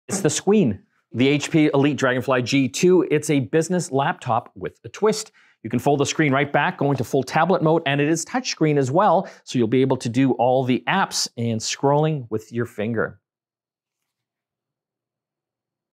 A Business Laptop With a Twist - HP Elite Dragonfly G2
The HP Elite Dragonfly G2 isn't your typical business notebook. It's lightweight, not heavy. It's sleek, not clumsy. And it's got a literal twist because it folds from your standard laptop to a tablet. We love the colour!

GetConnected brings you technology news, inspiring tech stories, and tips for digital living at home and work.